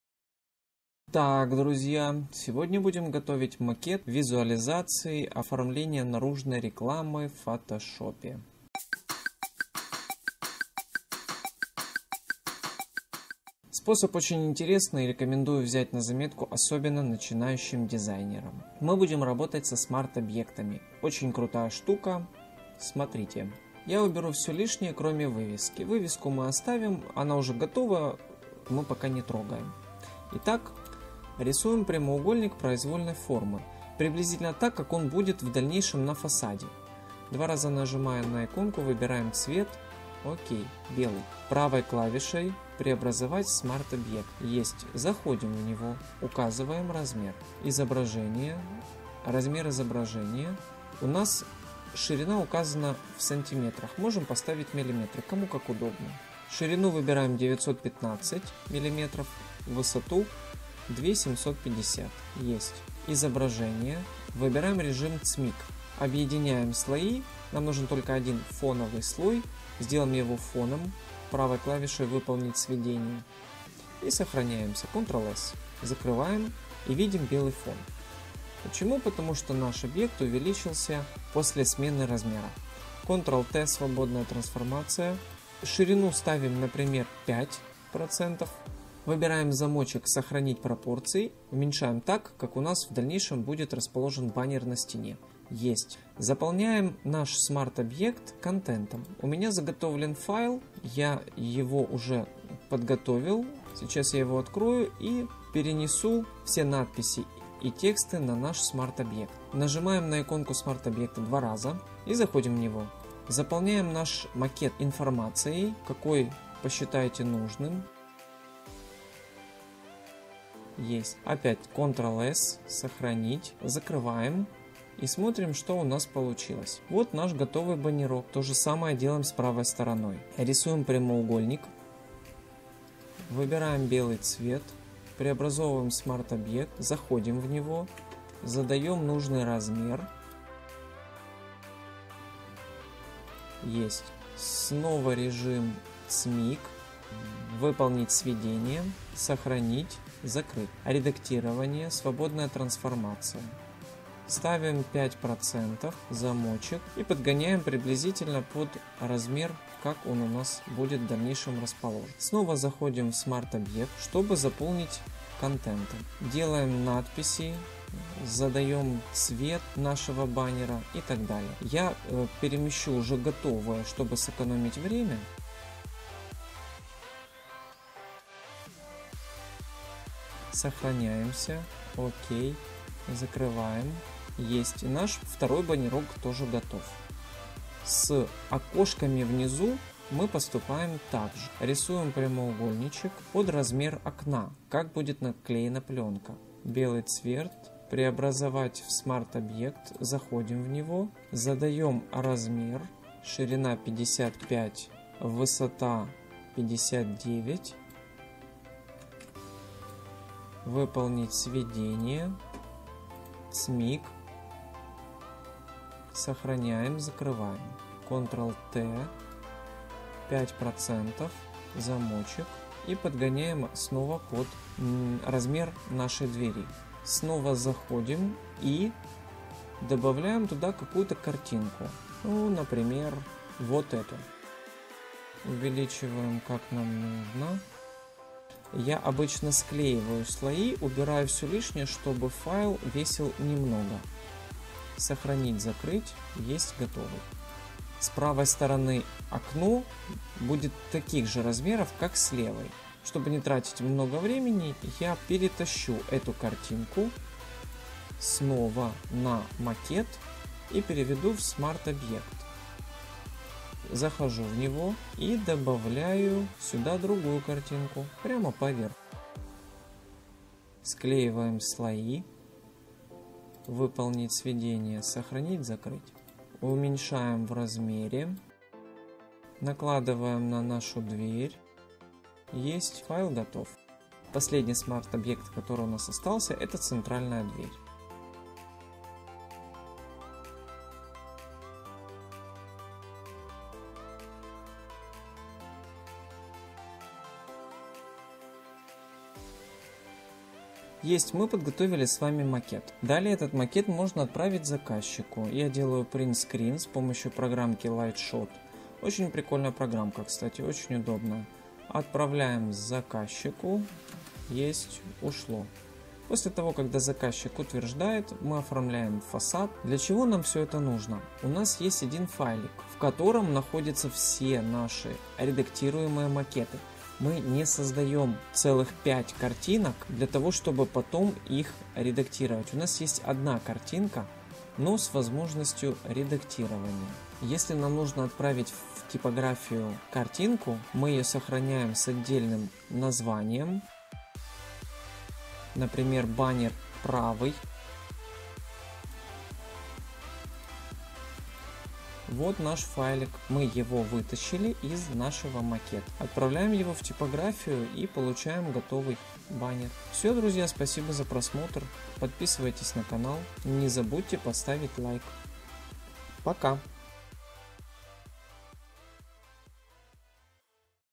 Как сделать МАКЕТ БАННЕРА в Фотошопе или Что такое СМАРТ ОБЪЕКТ в Photoshop
Рекламный макет в Фотошопе или Макет Баннера при помощи Смарт Объектов. В уроке я расскажу как сделать макет печати в Фотошопе. Smart object - это Очень удобный инструмент. С его помощью можно готовить конечный вид макета. При помощи готового mockup (шаблона). Сферы применения: полиграфия, листовки, баннера и многое другое. Смотрите уроки фотошоп для начинающих.